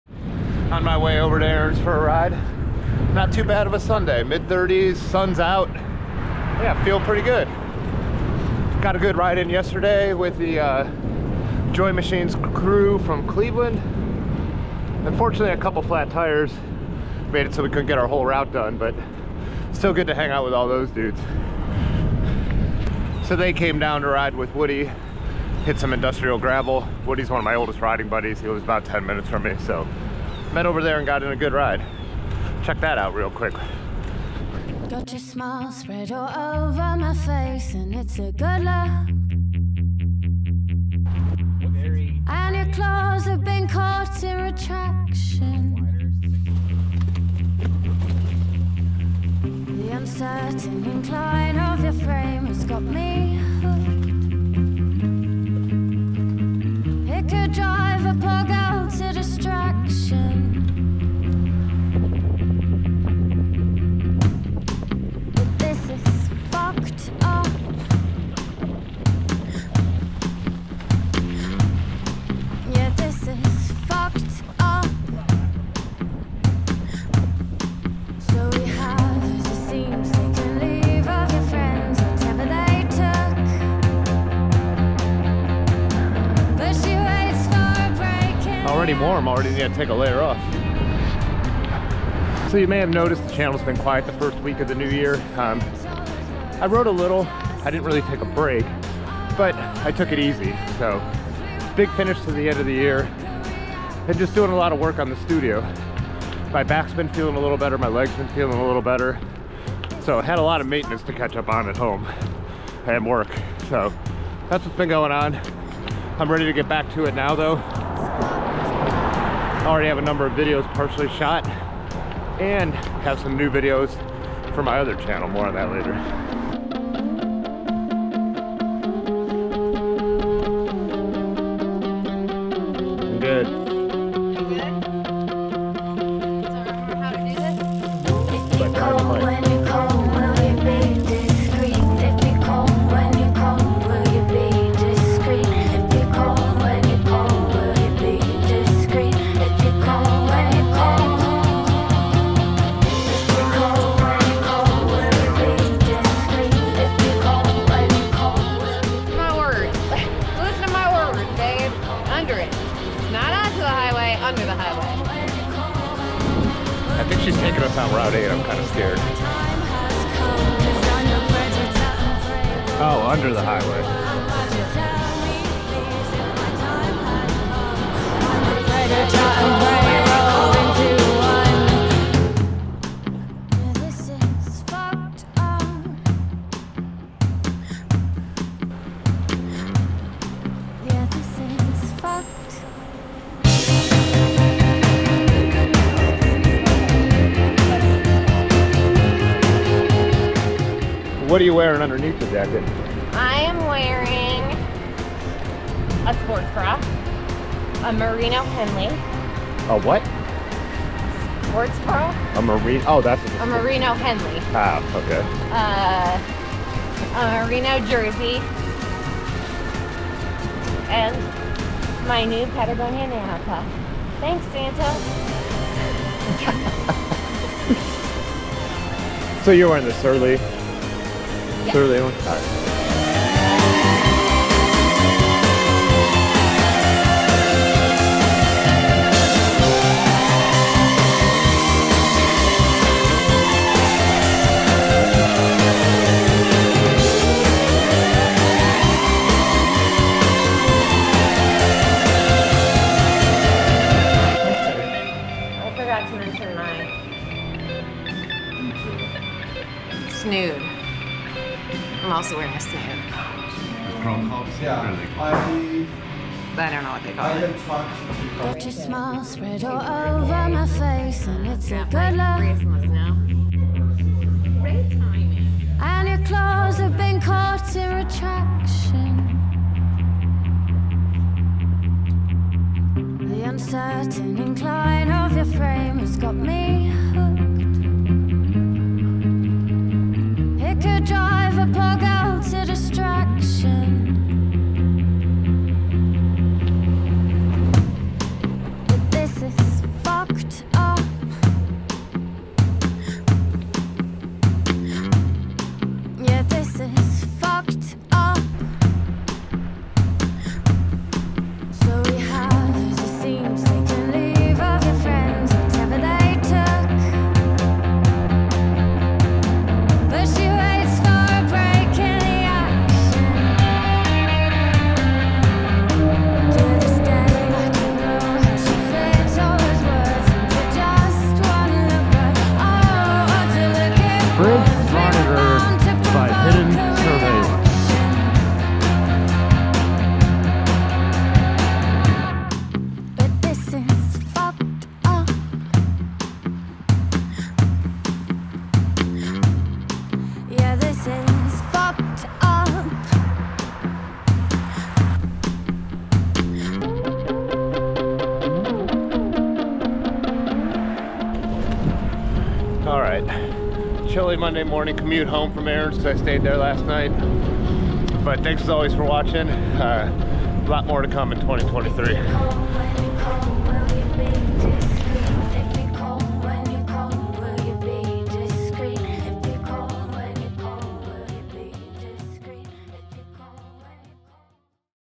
Winter Café Rides
The weekend rides. Some cruising with the Joy Machines Racing crew on Saturday - hitting some of the Akron cuts and Industrial Gravel, Then a Kent cruise and new route to the Hike & Bike with Erin on Sunday. A little chilly but a decent weekend. Not a ton a footage from the Joy Machines ride as some mechanicals really slowed us down and shortened that ride.